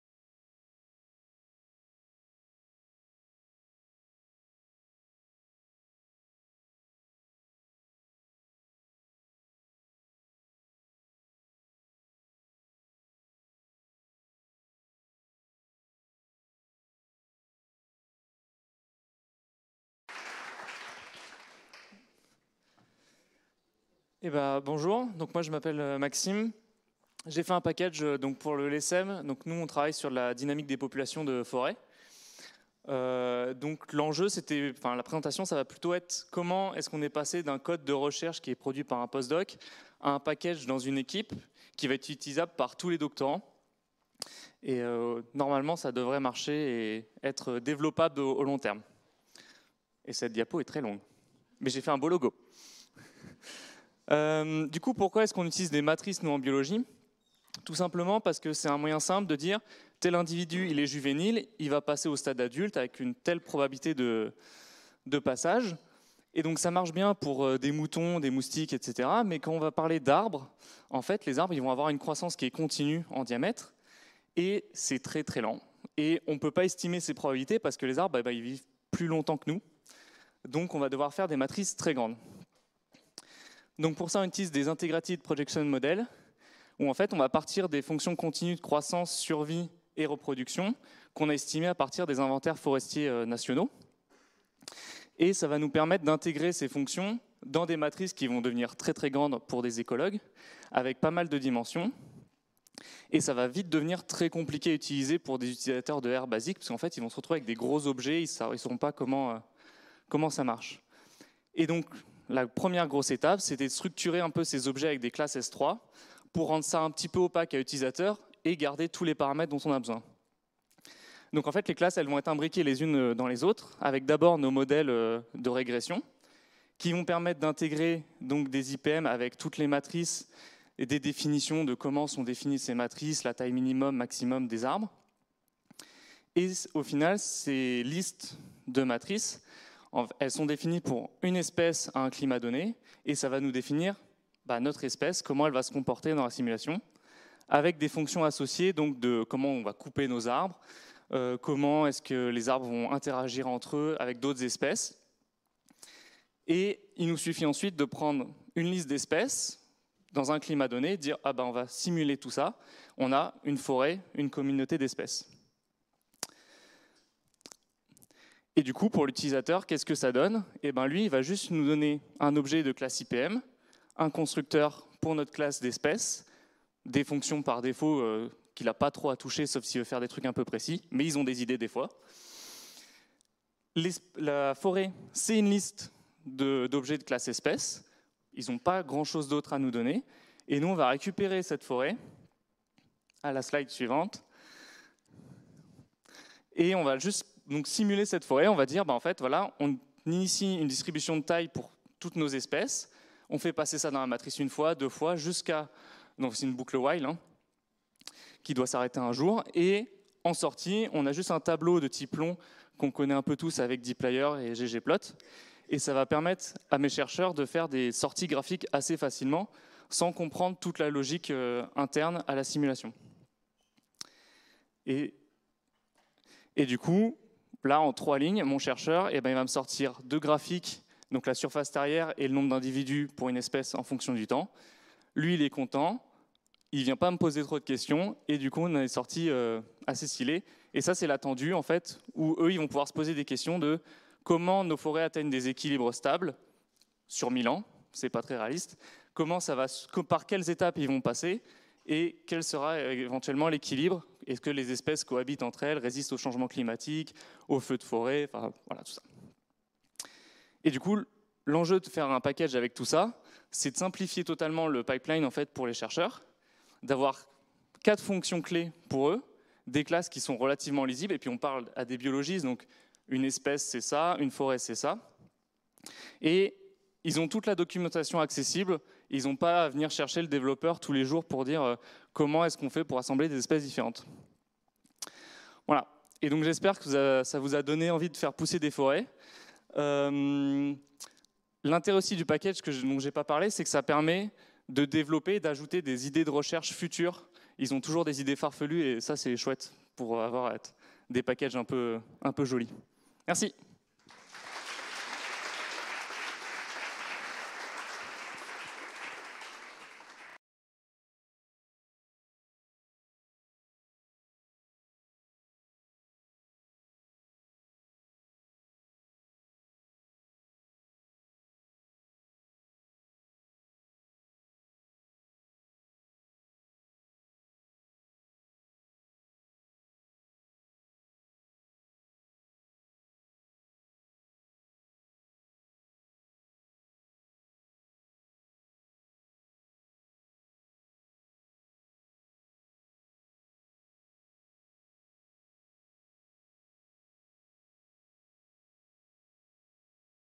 {matreex} : Simuler les dynamiques forestières européeennes - Maxime Jaunatre
{matreex} : Simuler les dynamiques forestières européeennes
Maxime Jaunatre, Laboratoire des EcoSystèmes et des Sociétés en Montagne

Captation : Service audiovisuel Avignon Université